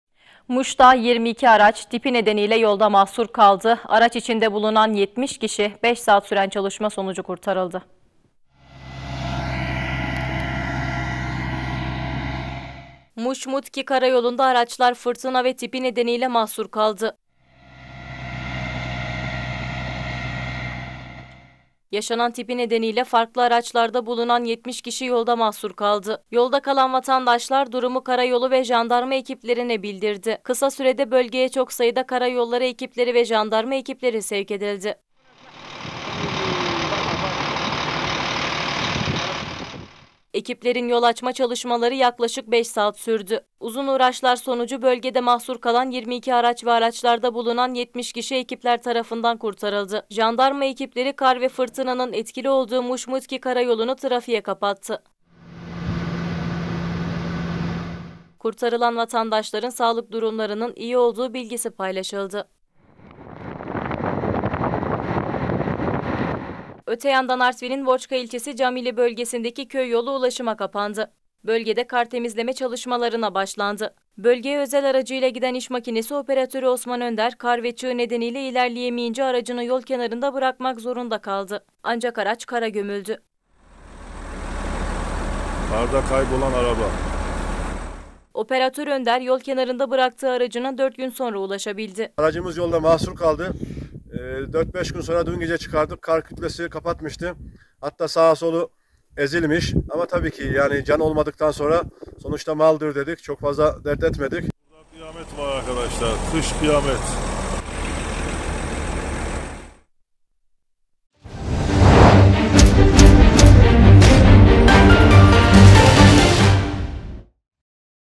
Kardelen TV | Muş'ta Karayolunda Mahsur Kalan 70 Kişi Kurtarıldı
Muş-Mutki karayolunda araçlar fırtına ve tipi nedeniyle mahsur kaldı. Yaşanan tipi nedeniyle farklı araçlarda bulunan 70 kişi yolda mahsur kaldı.